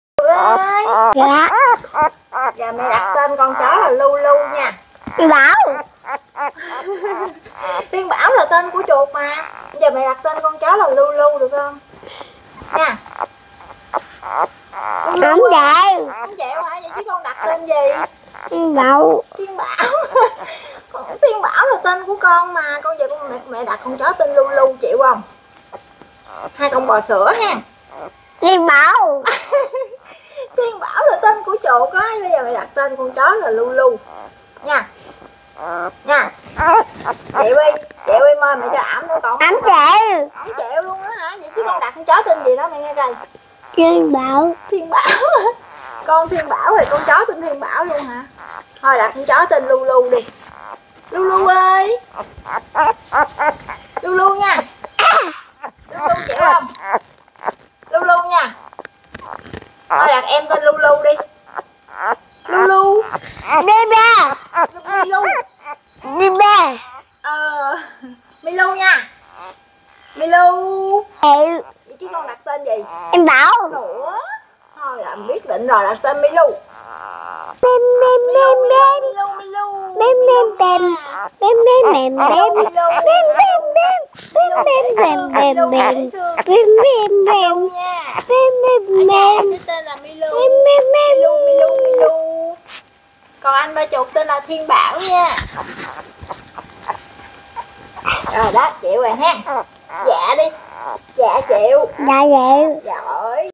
Chuột đặt tên cho bé chó sao mà mắc cười quá!! kkk
Chuột tên Thiên Bảo, mà giờ cũng muốn đặt tên cho bé chó là Thiên Bảo nửa,, nghe rối quá, mà đổi tên khác thì k chịu...khổ ghê..